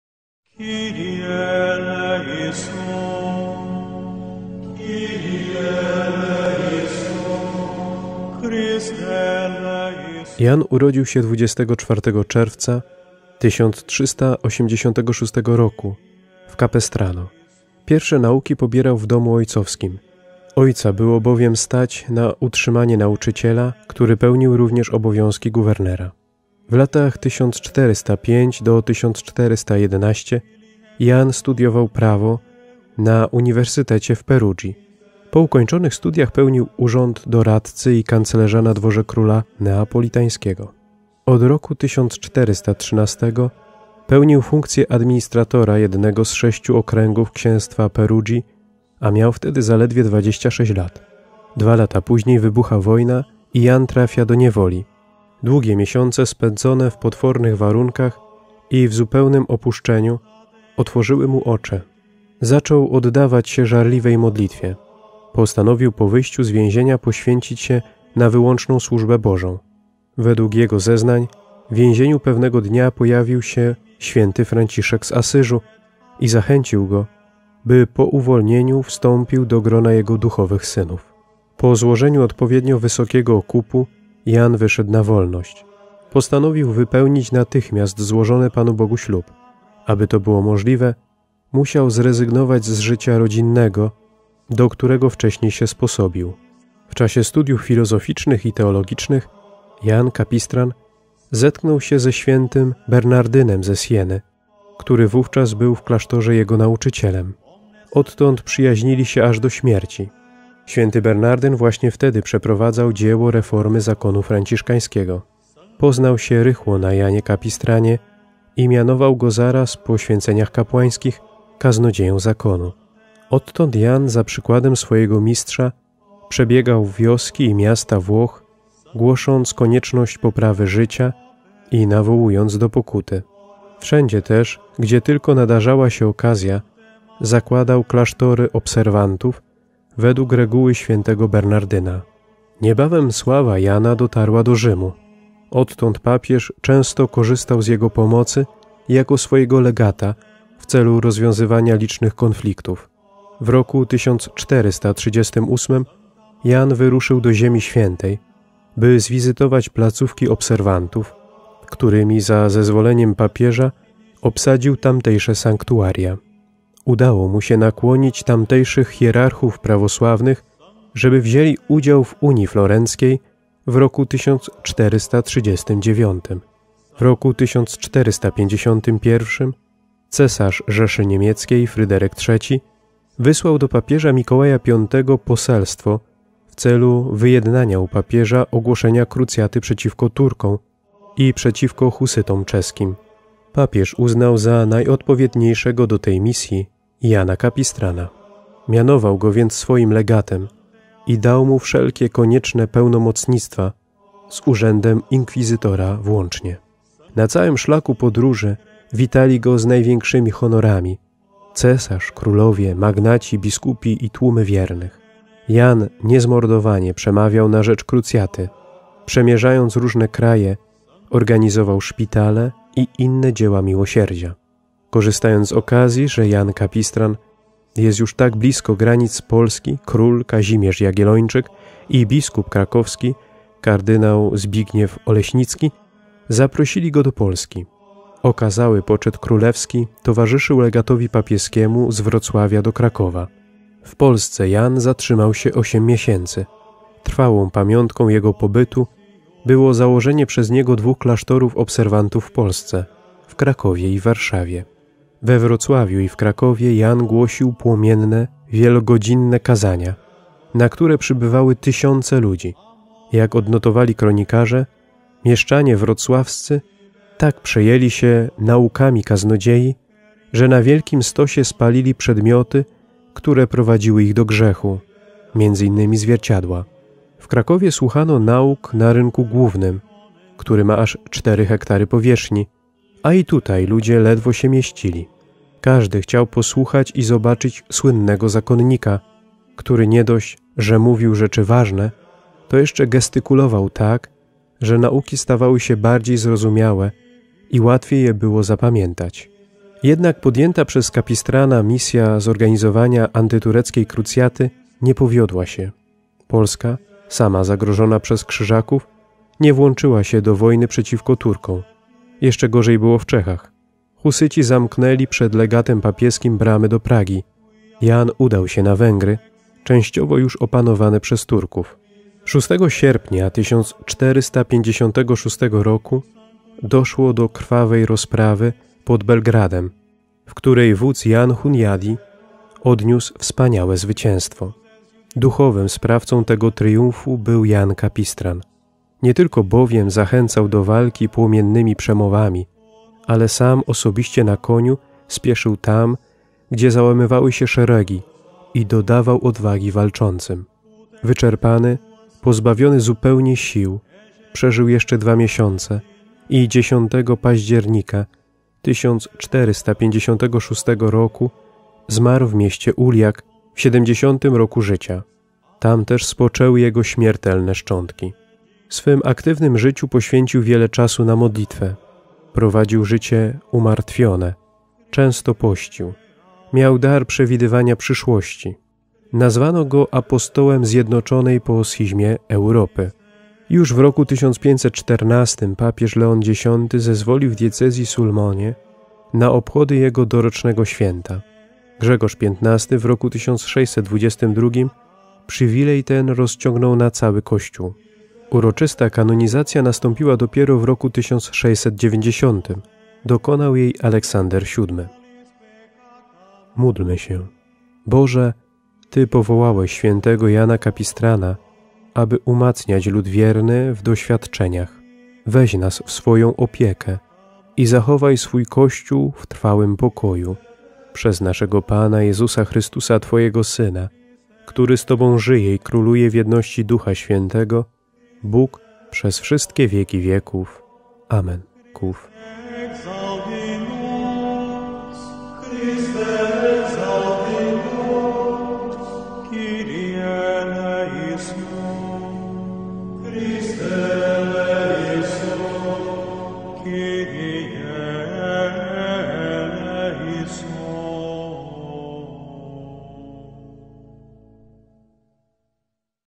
Skarby Kościoła 23 października | Św. Jan Kapistran
Patronem dnia 23 października jest św. Jan Kapistran - siewa jedności i pokoju w Europie.